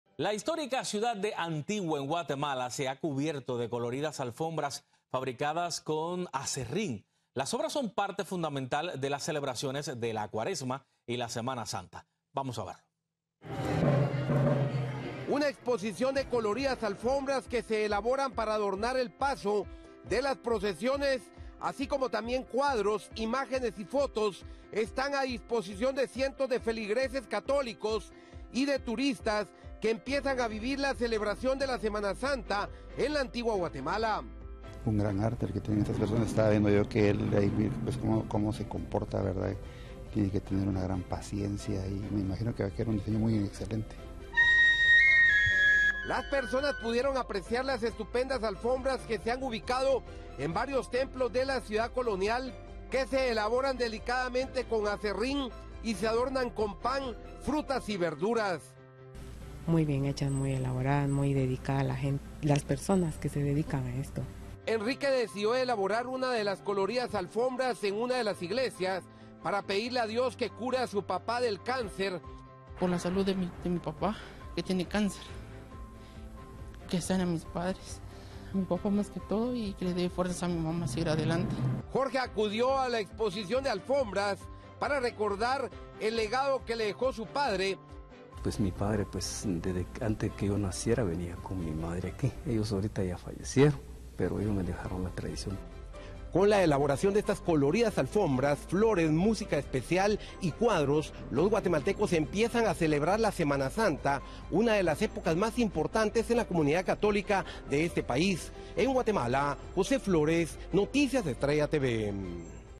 🇬🇹 Alfombras coloridas elaboradas con aserrín, una muestra de talento y cultura. |Buenas Noticias
🇬🇹 Una muestra de talento y cultura. Guatemala expone alfombras coloridas elaboradas con aserrín. Imágenes y fotos están a la disposición de feligreses católicos y turistas que empiezan a vivir la celebración de la semana santa en la Antigua Guatemala.